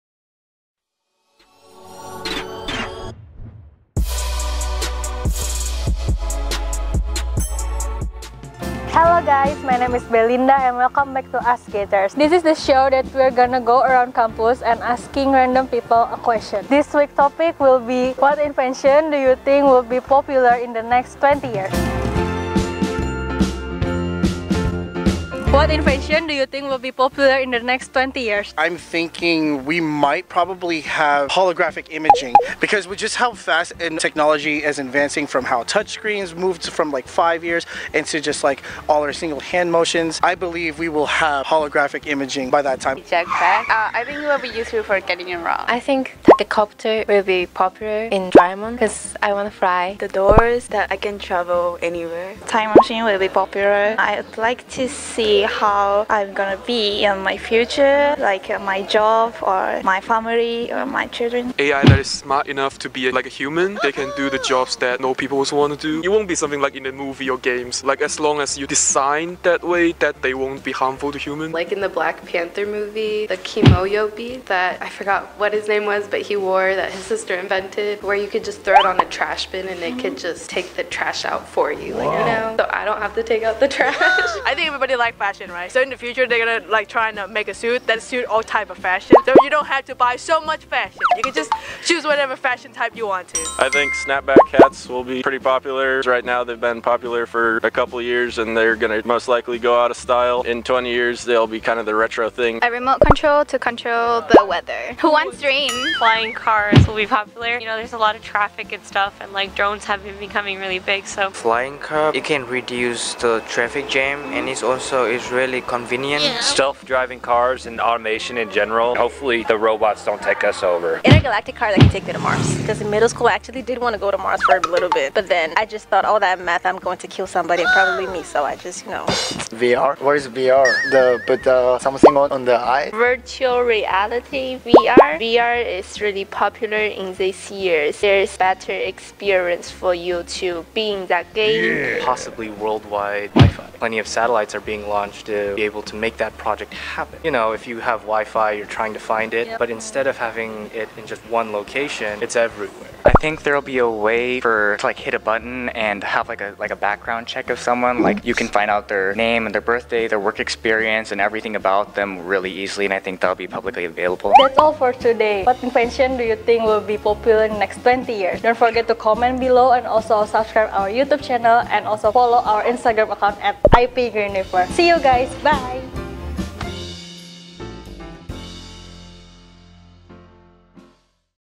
AskGators | EP.4 | What invention do you think will be popular in 20 years?
AskGators is a weekly show where we go around campus and ask students, faculty, and staff questions based on our weekly topic.

Today's topic is: What invention do you think will be popular in 20 years?